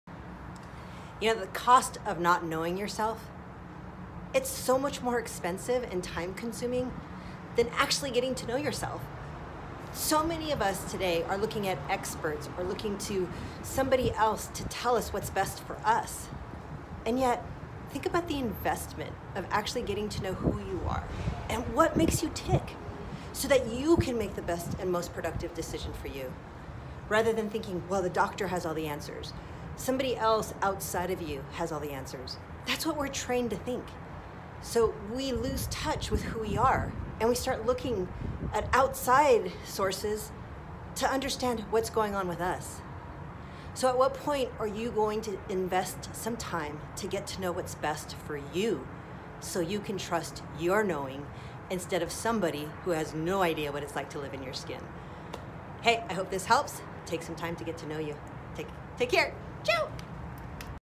Are You Willing to Invest in You?
We are taught to look to “experts” to know what is best for ourselves.

We will spend thousands of dollars attempting to fix ourselves based on what the experts say.

Yet, what if we simply invested in and on ourselves with TIME.

Have you invested time to get to know who you are?

What foods does your body respond well to?

How do you maneuver through challenges?

Can you change any unproductive habits?

STOP listening to the world and laying attention to not living up to its standards.

BEGIN to invest in and on you so YOU can know you better than any “expert”!